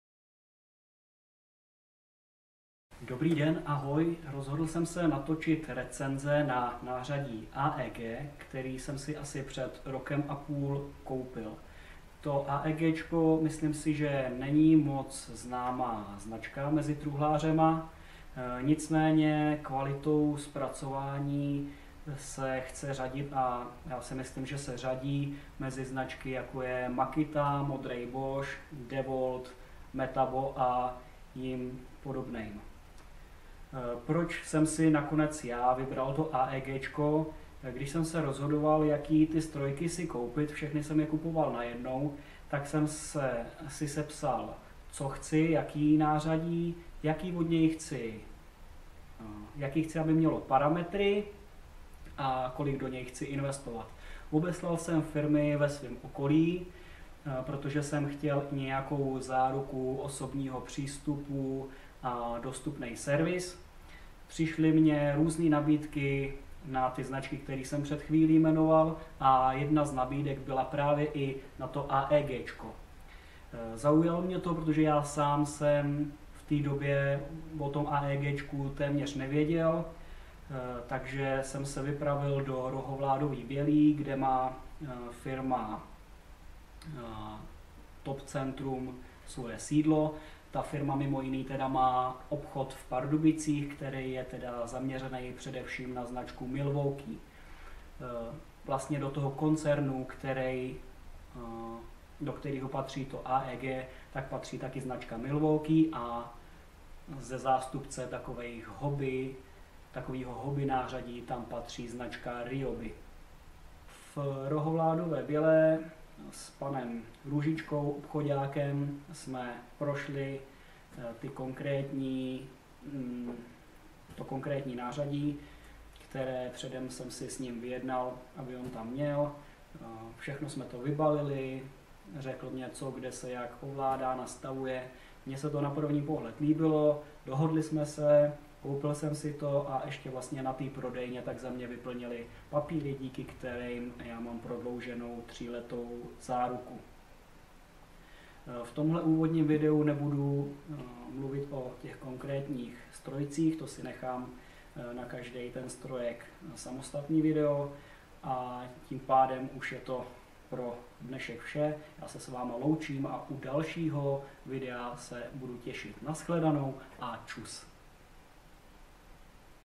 VENDA.work - Vendova recenze - Nářadí AEG
Pod dlouhé době jsem se dokopal, abych natočil video. Poprvé jsem zkusil recenzi. Jsem zvědavý, jaký bude mít ohlas.

\|E|\|D|\